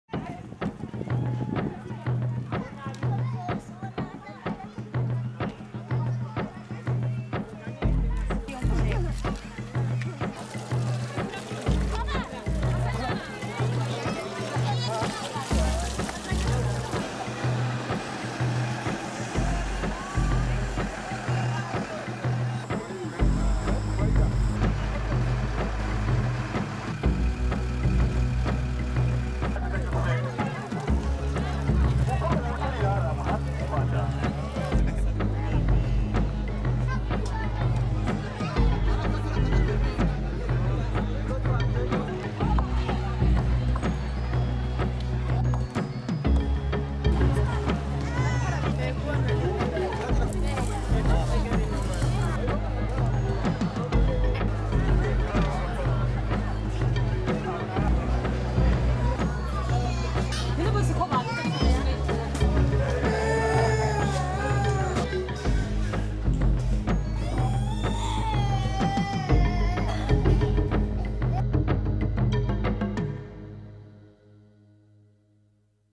Refugiados en Tanzania
[Tanzania]

El país alberga a más de 250,000 refugiados de Burundi y la República Democrática del Congo. Hoy en día, los campos están llenos. Esta es una de las crisis de refugiados más grandes de África.

La asistencia se debe incrementar con URGENCIA!